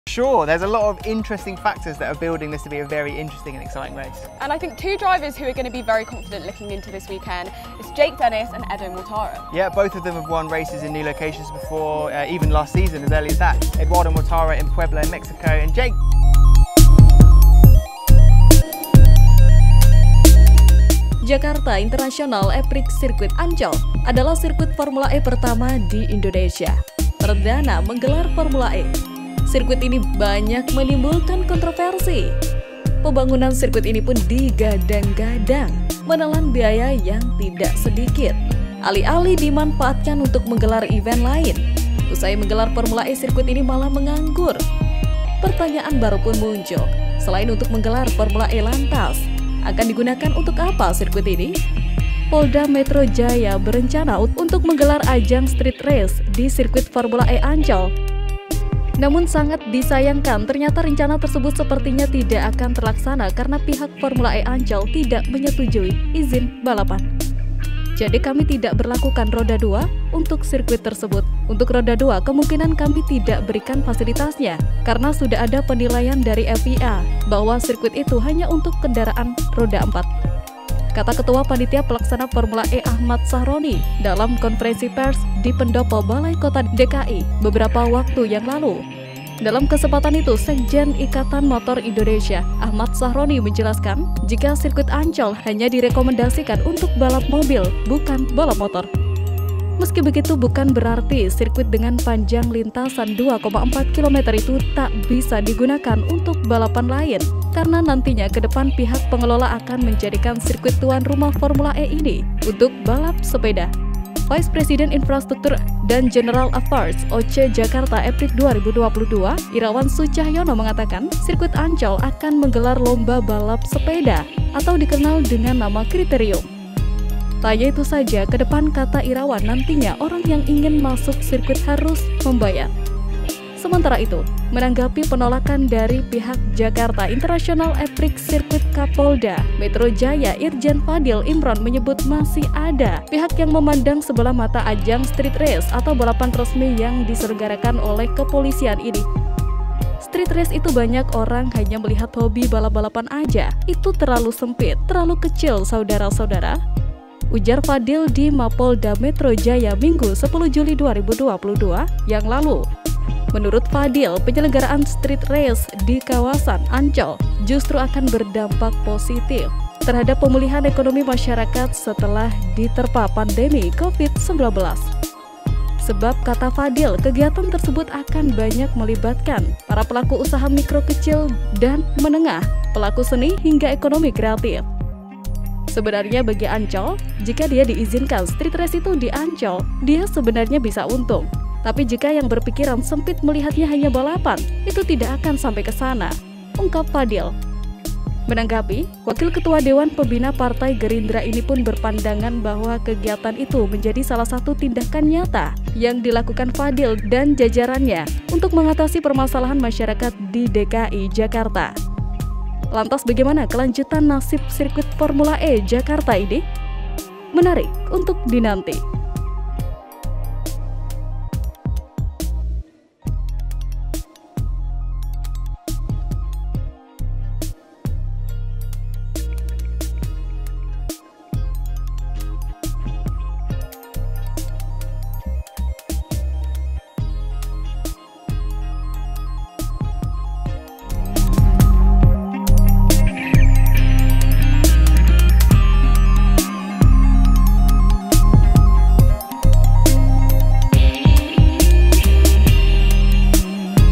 BUAT GEGER PUBLIK🔥SIRKUIT JAKARTA E-PRIX TERANCAM MENGANGGUR😱TAK BOLEH UNTUK GELARAN STREET RACE❗
BIKIN GEGER 🔥SIRKUIT JAKARTA E-PRIX TAK BOLEH DIPAKAI UNTUK STREET RACE❗ TERANCAM NGANGGUR 😱
TAK JADI GELAR DRAG RACE. JAKARTA INTERNATIONAL E-PRIX CIRCUIT NGANGGUR

Jakarta International E-Prix Circuit, Ancol adalah sirkuit formula E pertama di Indonesia. Perdana menggelar formula E, sirkuit ini banyak menimbulkan kontroversi. Pembangunan sirkuit ini pun digadang – gadang menelan biaya yang tidak sedikit. Alih – alih dimanfaatkan untuk menggelar event lain, usai menggelar formula E sikruit ini malah menganggur. Pertanyaan baru pun muncul, selain untuk menggelar formula E, lantas akan digunakan untuk apa sirkuit ini? Polda Metro Jaya berencana untuk menggelar ajang Street Race di Sirkuit Formula E Ancol. Namun sangat disayangkan, ternyata rencana tersebut sepertinya tidak akan terlaksana karena pihak Formula E Ancol tidak menyetujui izin balapan. "Jadi kami tidak berlakukan roda dua untuk sirkuit tersebut. Untuk roda dua kemungkinan kami tidak berikan fasilitasnya, karena sudah ada penilaian dari FIA bahwa sirkuit itu hanya untuk kendaraan roda empat," kata Ketua Panitia Pelaksana Formula E Ahmad Sahroni dalam konferensi pers di Pendopo Balai Kota DKI, beberapa waktu yang lalu.
Dalam kesempatan itu, Sekjen Ikatan Motor Indonesia Ahmad Sahroni menjelaskan jika Sirkuit Ancol hanya direkomendasikan untuk balap mobil, bukan balap motor. Meski begitu, bukan berarti sirkuit dengan panjang lintasan 2,4 km itu tak bisa digunakan untuk balapan lain. Karena nantinya ke depan pihak pengelola akan menjadikan sirkuit tuan rumah Formula E ini untuk balap sepeda. Vice President Infrastructure & General Affairs OC Jakarta EPrix 2022 Irawan Sucahyono mengatakan, Sirkuit Ancol akan menggelar lomba balap sepeda atau dikenal dengan nama criterium. Tak hanya itu sjaa, ke depan kata Irawan, nantinya orang yang ingin masuk sirkuit harus membayar.
Sementara itu, menanggapi penolakan dari pihak Jakarta International E-Prix Circuit, Kapolda Metro Jaya Irjen Fadil Imran menyebut masih ada pihak yang memandang sebelah mata ajang street race atau balapan resmi yang diselenggarakan oleh kepolisian ini. "Street race itu banyak orang hanya melihat hobi balap-balapan saja. Itu terlalu sempit, terlalu kecil saudara-saudara," ujar Fadil di Mapolda Metro Jaya, Minggu (10/7/2022) yang lalu.